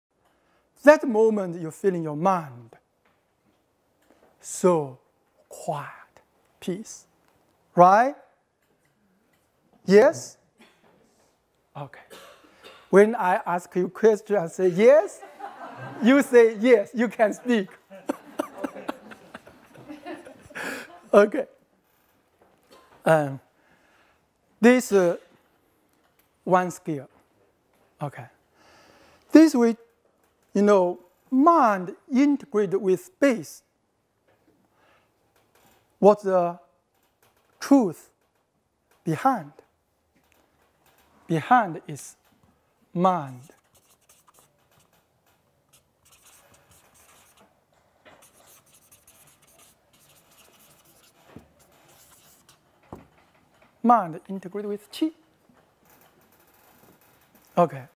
Master Yuantong Liu (3)
Mind integrates with Qi
Mind integrated with space, what is the truth behind? Mind integrates with Qi. Because the Qi is formless, living within the space. Feel the space between you and me. Focus the mind in the space between you and me and use your sensory ability of the whole body to feel this space and connection. When you feel your mind meeting with the space between us you feel your heart and mind naturally calm and quiet. That quietness, calmness will bring the mind entering the basic state of relaxation. When the mind is in this state, the nerve system of brain of our body will naturally relax as well. Our consciousness activity is so strongly impacting the nerves activity of brain and body.

This lecture will be continued tomorrow!
Hun Yuan Ling Tong

Mente integrada con Qi
Una vez que la mente se integra con el espacio, cual es la siguiente verdad? La mente se integra con el Qi. El Qi presente en el espacio no tiene forma. Siente el espacio entre tu y yo. Enfoca la mente en el espacio entre nosotros, y usa las habilidades sensoriales de todo el cuerpo para sentir este espacio y la conexión. Al sentir que la mente se encuentra con el espacio entre nosotros, tambien sentirás tu corazón y tu mente naturalmente serenos y en calma. Esa serenidad y calma hacen que la mente entre en el estado básico de relajación. Con la mente en este estado, el sistema nervioso, el cerebro y el cuerpo también se relajan naturalmente. Nuestras actividades conscientes tienen un impacto muy fuerte en la actividad de los nervios del cerebro y de todo el cuerpo.

Esta conferencia se continuará mañana!

to see the whole 8-day workshop with Master Yuantong Liu